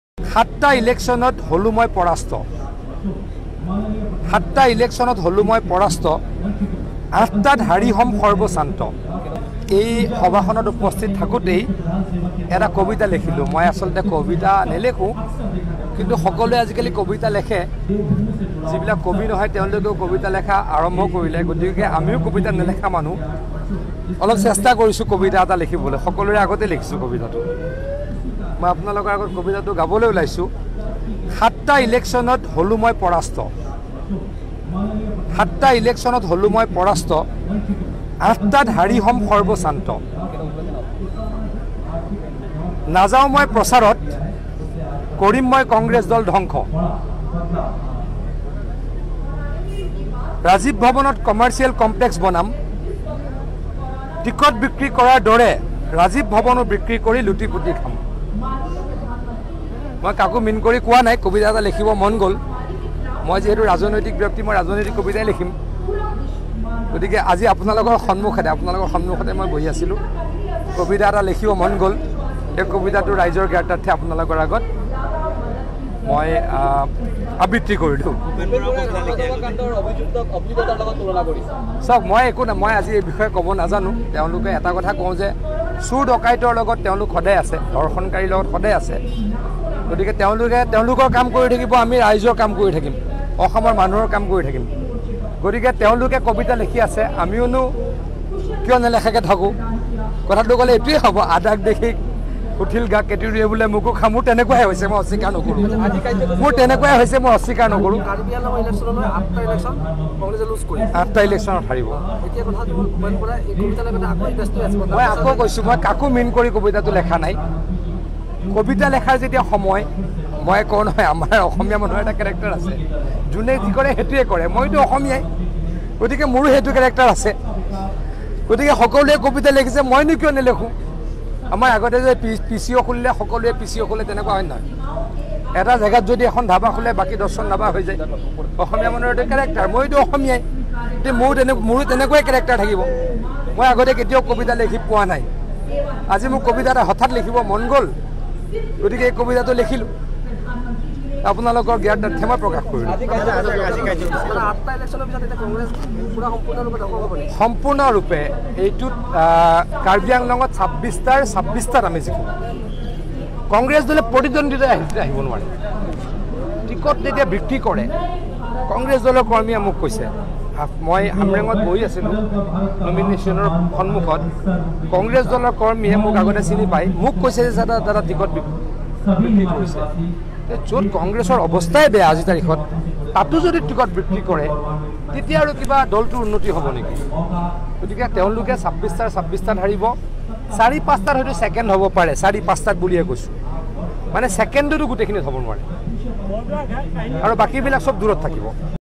PijushHazarika Minister Assam Poem Kabita BhupenBora Congress Apcc
এইবাৰ কবি হ’ল মন্ত্ৰী পীযূষ হাজৰিকা। নাম প্ৰকাশ নকৰাকৈ কবিতাৰে ভূপেন বৰাক কটাক্ষ পীযূষ হাজৰিকাৰ।